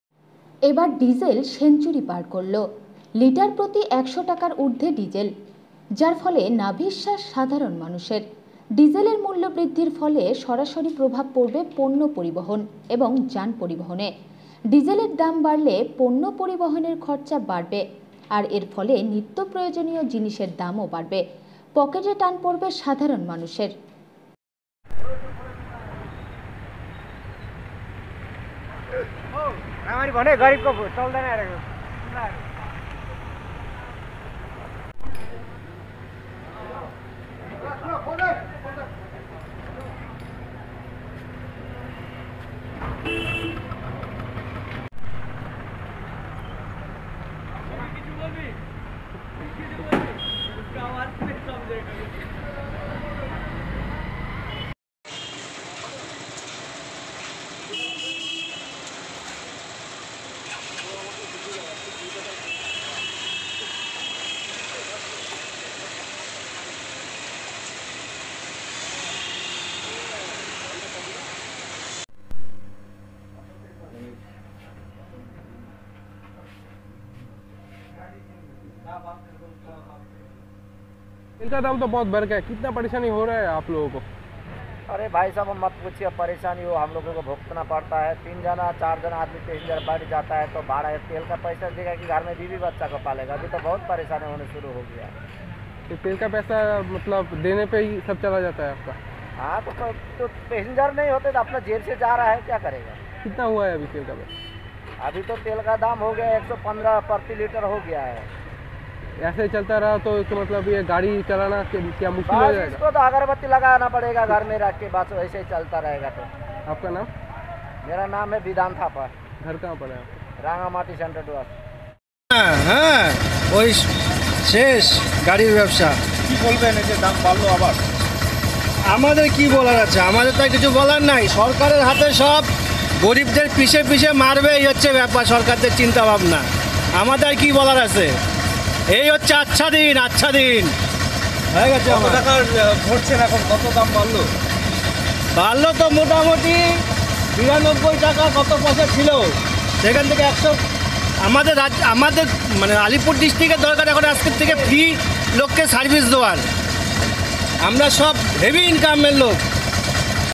এবার ডিজেল সেঞ্চুরি পার করলো।
এবার ডিজেল সেঞ্চুরি পার করলো। লিটার প্রতি 100 টাকার ঊর্ধে ডিজেল, যার ফলে নাভিশ্বাস সাধারণ মানুষের। ডিজেলের মূল্য বৃদ্ধির ফলে সরাসরি প্রভাব পড়বে পণ্য পরিবহন এবং যান পরিবহনে। ডিজেলের দাম বাড়লে পণ্য পরিবহনের খরচা বাড়বে আর এর ফলে নিত্য প্রয়োজনীয় জিনিসের দাম বাড়বে,পকেট টান পড়বে সাধারণ মানুষের।গতকাল ডিজেল এর দাম ৯৯.৩৪ টাকা আজকে গিয়ে দাঁড়ালো ১০০.১৪ টাকা  গতকাল পেট্রোল এর দাম ছিল ১১৪.৫৬ টাকা আজকে গিয়ে দাঁড়ালো ১১৫.৪৯ টাকা এক্সট্রা প্রিমিয়াম পেট্রোল গতকাল দাম ছিল ১১৯.৫১টাকা আজকে গিয়ে দাড়ালো ১২০.৭৭টাকা